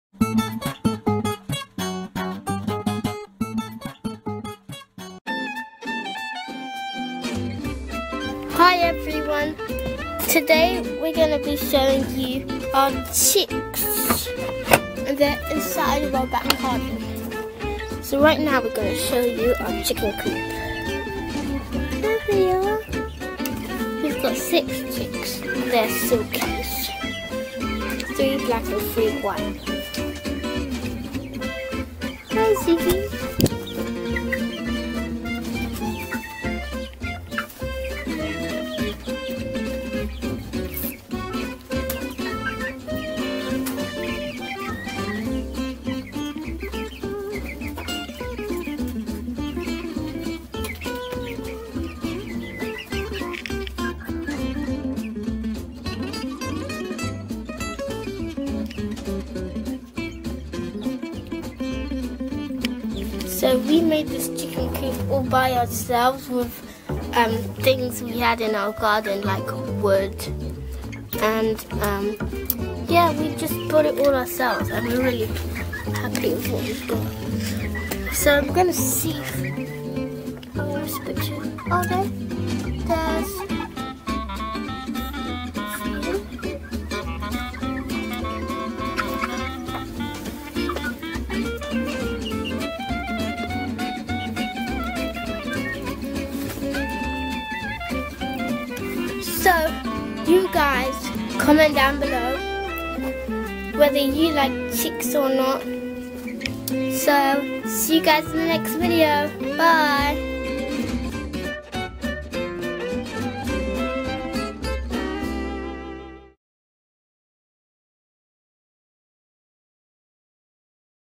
Meet Our CHICKS!!!
These are our chicks! They are Silkies and are friendly but eat more than you can imagine!  XD     We don't have them eating in this video but we will try to in future chick videos!